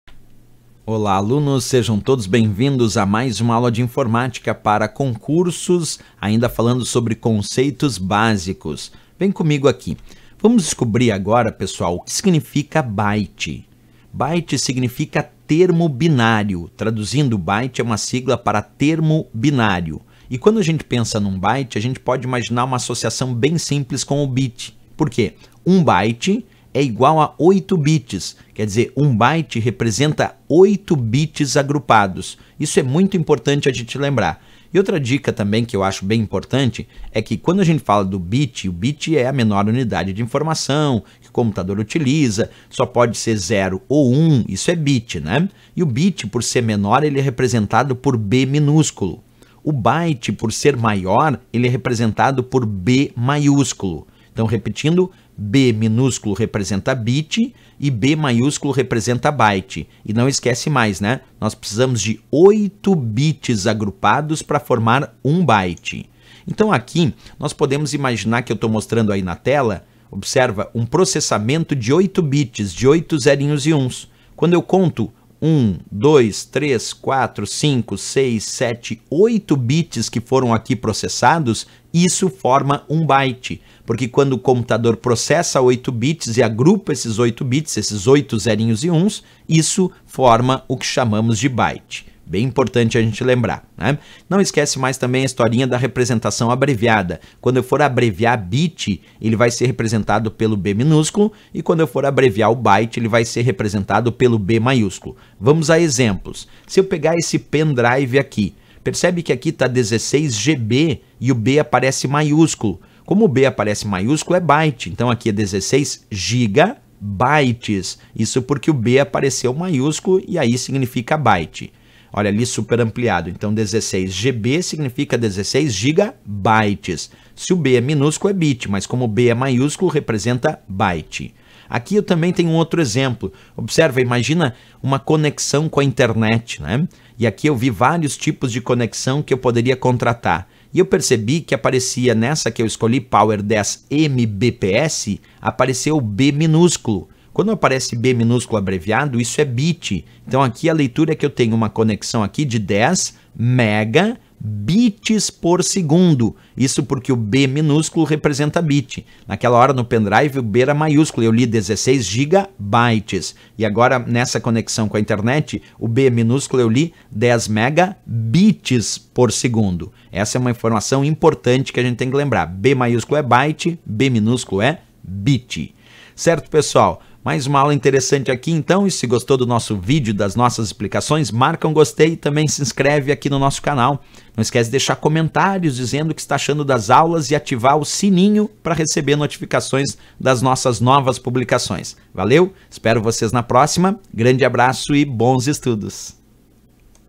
Conceitos Básicos de Informática para Concursos 2019 - Aula 8
Nesta videoaula sobre conceitos básicos de informática para concursos o professor Rodrigo Schaeffer fala sobre a diferença entre bit e byte.